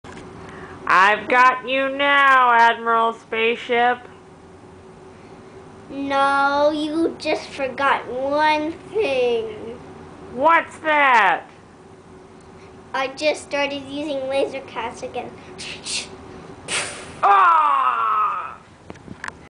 Mini Laser Cats
Ellie slighty recreating laser cats.